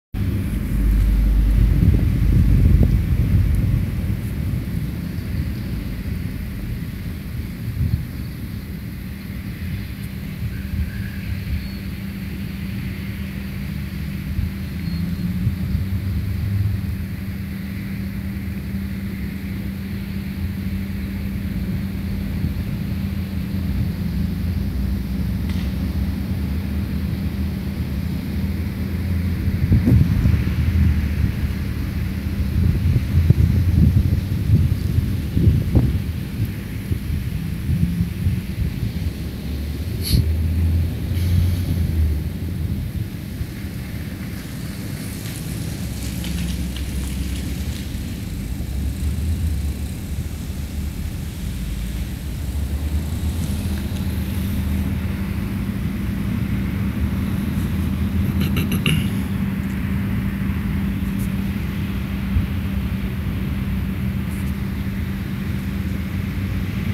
Tcl Idol X S950 пример видео Full HD
Пример видео съемки смартфона TCL idol x s950 в формате Full HD (1920*1080 точек)